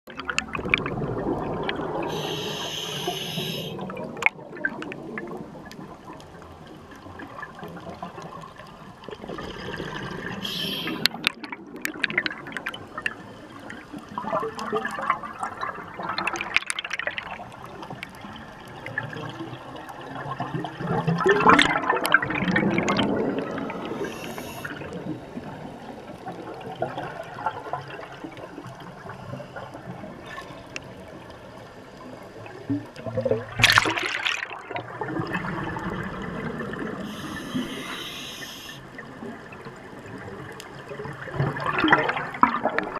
Katie's bubbles
I swim about 10 feet above Katie and film her bubbles.